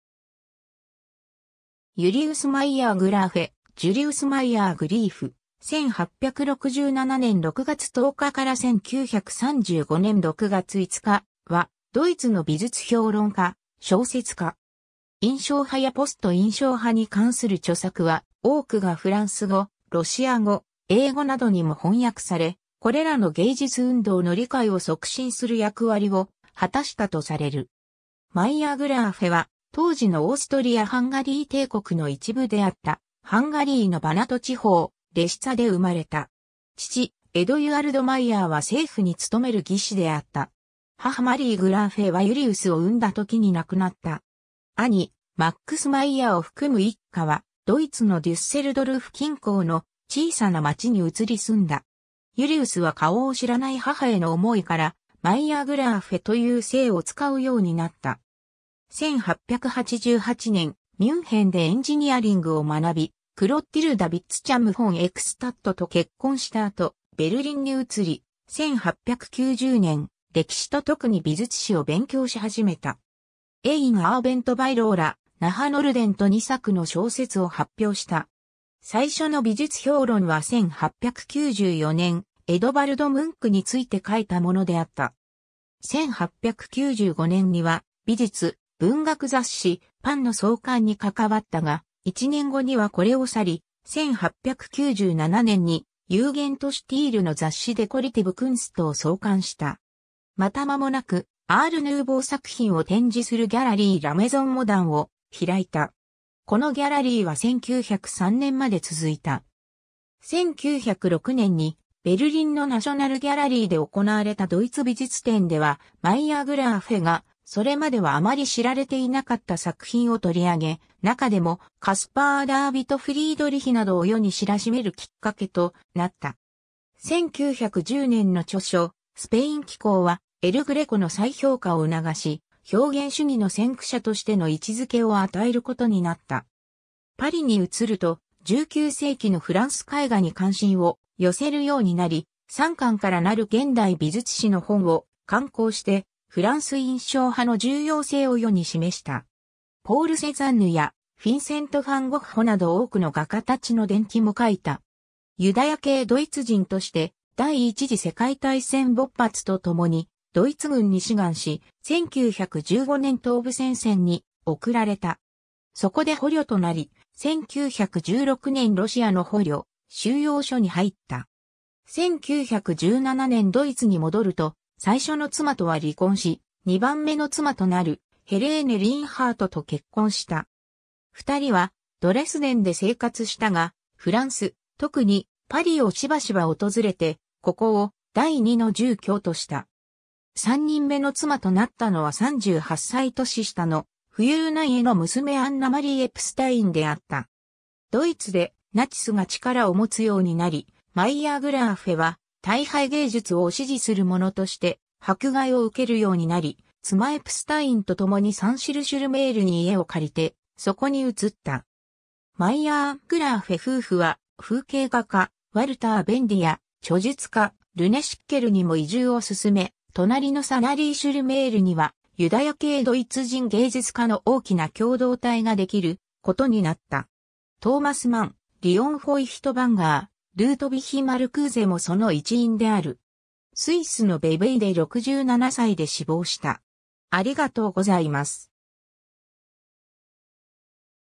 ユリウス・マイヤー＝グラーフェ
ユリウス・マイヤー＝グラーフェ (Julius Meier-Graefe, 1867年6月10日 - 1935年6月5日）は、ドイツの美術評論家・小説家。
印象派やポスト印象派に関する著作は、多くがフランス語、ロシア語、英語などにも翻訳され、これらの芸術運動の理解を促進する役割を果たしたとされる。
 マイヤー＝グラーフェは、当時のオーストリア＝ハンガリー帝国の一部であったハンガリーのバナト地方レシツァ（現在のルーマニア）で生まれた。
父エドュアルド・マイヤーは政府に勤める技師であった。
母マリー・グラーフェはユリウスを産んだ時に亡くなった。
兄マックス・マイヤーを含む一家はドイツのデュッセルドルフ近郊の小さな町に移り住んだ。
ユリウスは顔を知らない母への思いから、マイヤー＝グラーフェという姓を使うようになった。
 1888年、ミュンヘンでエンジニアリングを学び、Clotilde Vitzthum von Eckstädtと結婚した後、ベルリンに移り、1890年、歴史と特に美術史を勉強し始めた。
"Ein Abend bei Laura"（1890年）、"Nach Norden"（1893年）と2作の小説を発表した。
最初の美術評論は1894年、エドヴァルド・ムンクについて書いたものであった。
1895年には美術・文学雑誌『パン』の創刊に関わったが、1年後にはこれを去り、1897年にユーゲント・シュティール（アール・ヌーヴォー）の雑誌Dekorative Kunstを創刊した。
また間もなくアール・ヌーヴォー作品を展示するギャラリーLa Maison Moderneを開いた。
このギャラリーは1903年まで続いた。
 1906年にベルリンのナショナル・ギャラリーで行われたドイツ美術展では、マイヤー＝グラーフェが、それまでは余り知られていなかった作品を取り上げ、中でもカスパー・ダーヴィト・フリードリヒなどを世に知らしめるきっかけとなった。
1910年の著書『スペイン紀行』はエル・グレコの再評価を促し、表現主義の先駆者としての位置づけを与えることになった。
 パリに移ると、19世紀のフランス絵画に関心を寄せるようになり、3巻から成る現代美術史の本（1904年、1914年-24年）を刊行してフランス印象派の重要性を世に示した。
ポール・セザンヌやフィンセント・ファン・ゴッホなど多くの画家たちの伝記も書いた。
 ユダヤ系ドイツ人として、第一次世界大戦勃発とともにドイツ軍に志願し、1915年東部戦線に送られた。
そこで捕虜となり、1916年ロシアの捕虜収容所に入った。
1917年ドイツに戻ると、最初の妻とは離婚し、2番目の妻となるHelene Lienhardtと結婚した。
2人はドレスデンで生活したが、フランス、特にパリをしばしば訪れて、ここを第2の住居とした。
 3人目の妻となったのは38歳年下の、富裕な家の娘アンナ・マリー・エプスタイン (Anna Marie Epstein) であった。
ドイツでナチスが力を持つようになり、マイヤー＝グラーフェは「退廃芸術」を支持する者として迫害を受けるようになり、妻エプスタインとともにサン＝シル＝シュル＝メールに家を借りてそこに移った。
マイヤー＝グラーフェ夫婦は風景画家Walter Bondyや著述家René Schickeleにも移住を勧め、隣のサナリー＝シュル＝メールにはユダヤ系ドイツ人芸術家の大きな共同体ができることになった。
トーマス・マン、リオン・フォイヒトヴァンガー、ルートヴィヒ・マルクーゼもその一員である。
 スイスのヴェヴェイで67歳で死亡した。